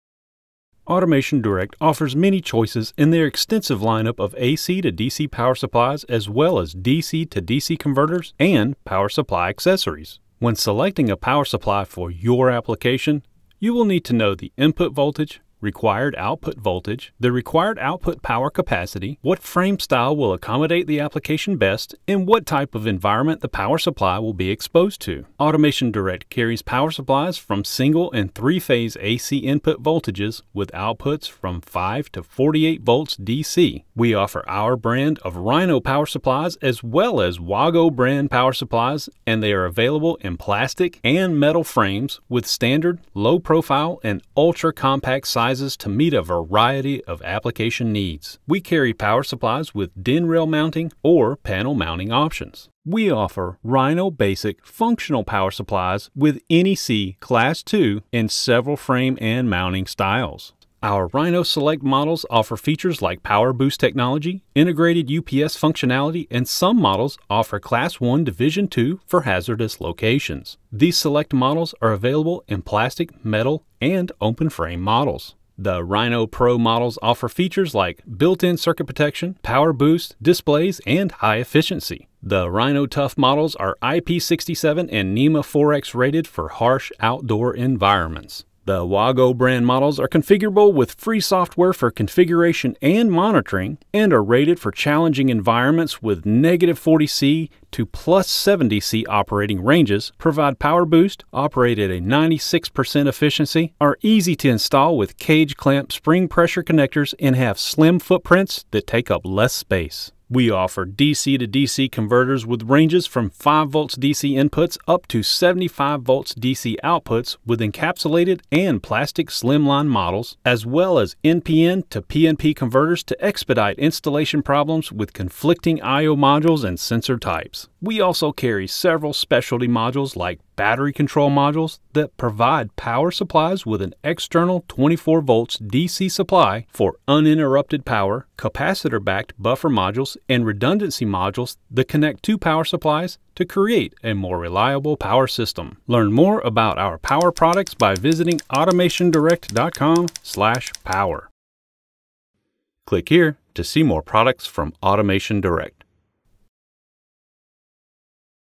Power Supply & DC-to-DC Converter Overview from AutomationDirect
In this video, we highlight the power products with several hundred power supplies and DC-to-DC converters. How to select a power supply. What to do if you need to convert DC power to a different DC voltage. Programming a power supply. AutomationDirect offers a wide range of power supplies in different voltages, amperages, frame styles, and brands.